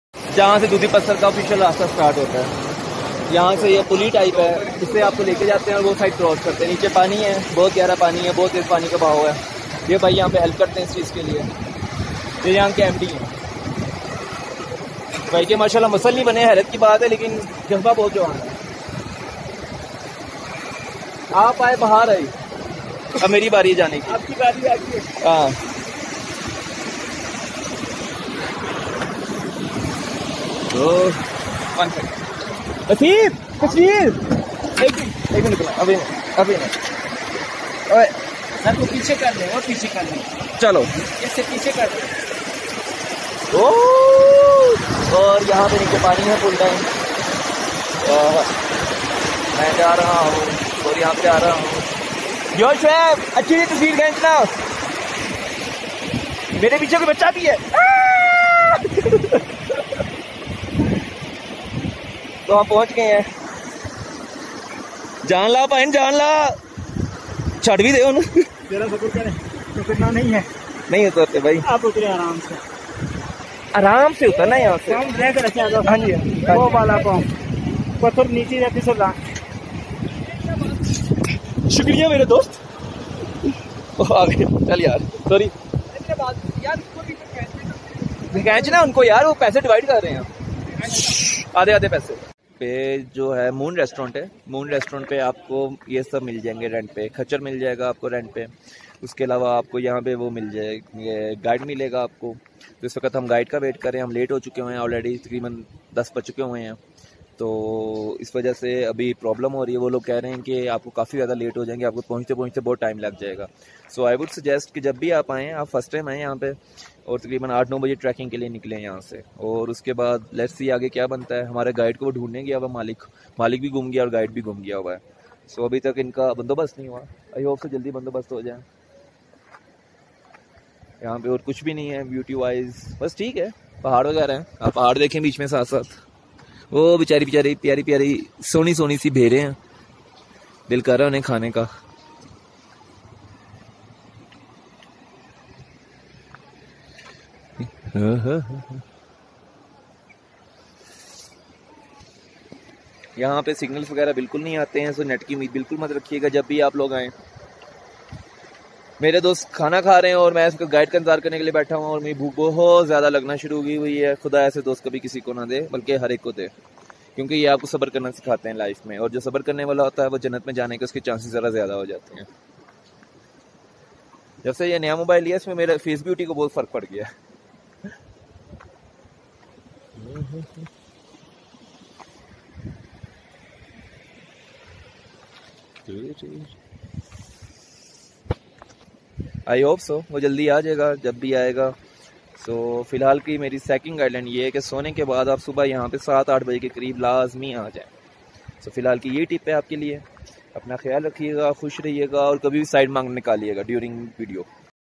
World best trekking - Dudipatsar - Naran Kaghan- Trekking towards DudiPatsar- Dudipatsar Lake Part 1
Video is not aligned well but it's just a starting point for the trekking . . so let it be . . :) :)